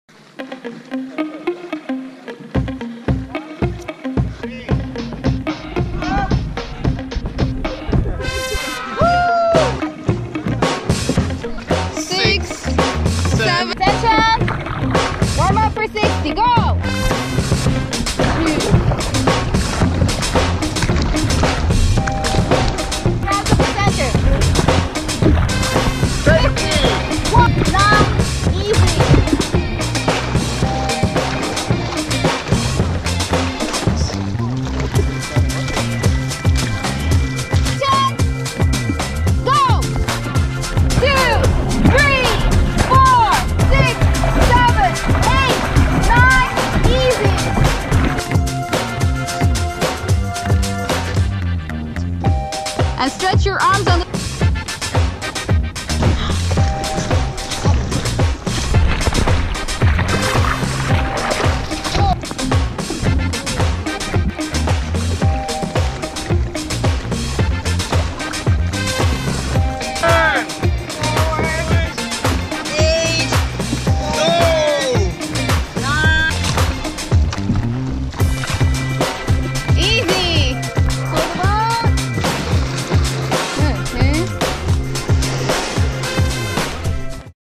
Dragon boating in Qatar -The Doha Wireless Warriors
The Doha Wireless Warriors
Dragon Boat Team for Cancer Survivors and Supporters